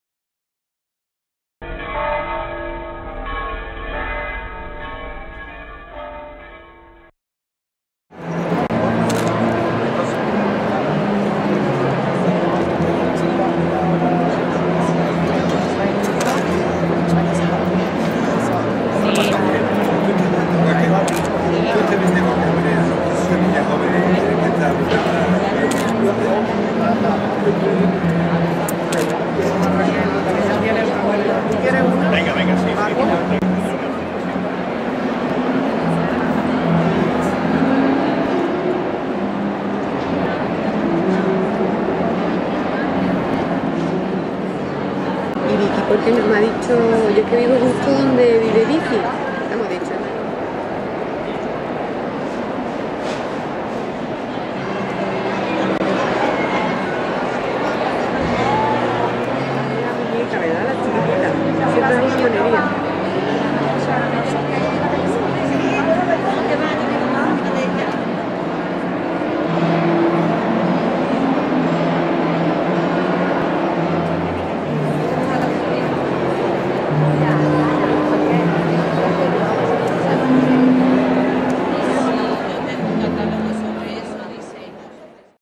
Simof 2010 - Inauguración
El jueves 28 de enero de 2010 se inauguró en Sevilla la XVI edición de Simof